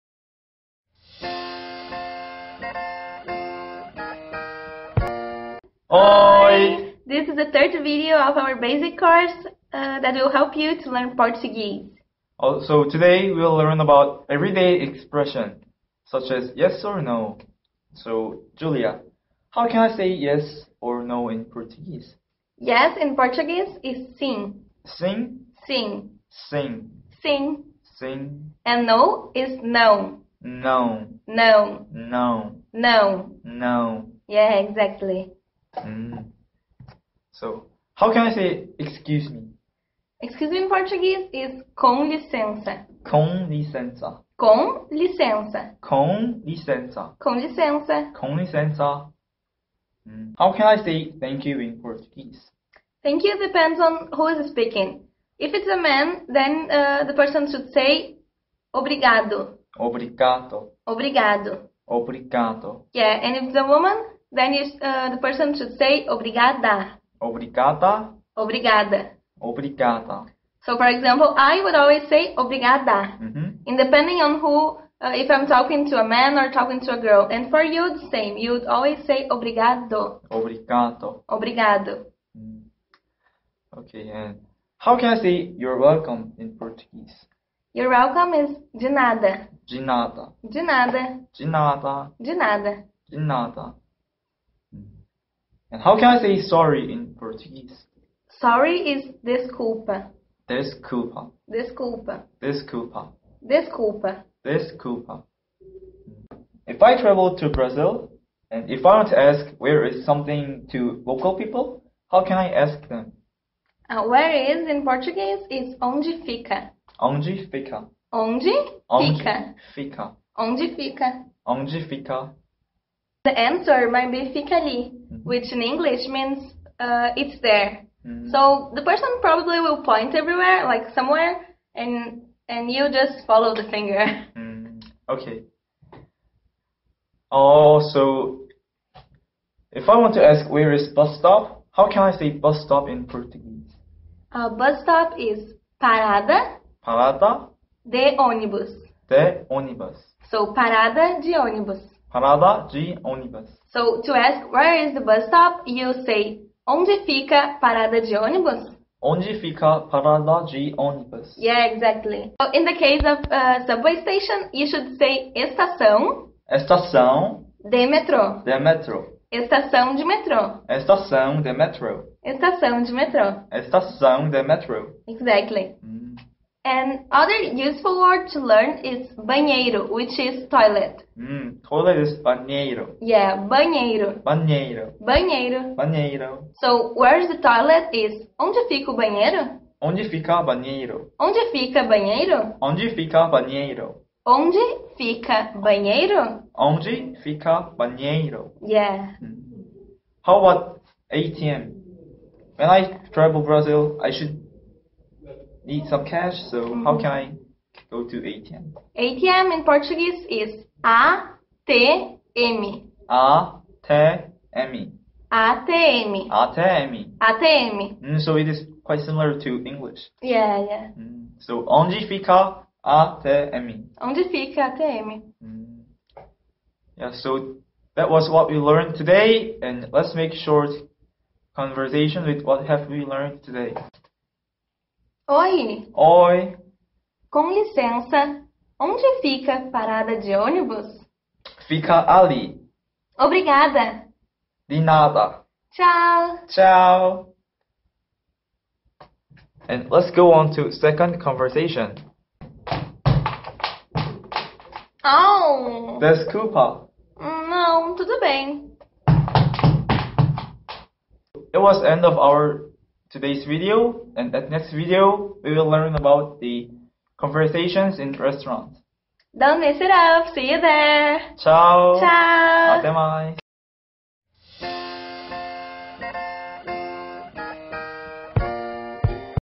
Portuguese 101 - Lesson 3 (Daily Expressions) 포르투갈어 배워보기 - 일상회화편
Oi!!
This is the third lesson of Portuguese 101 course, the fastest and easiest way to learn Portuguese.
We provide korean subtitles in the videos, so don´t forget to turn on your subtitles option!
Give us comment with suggestions for the next videos :)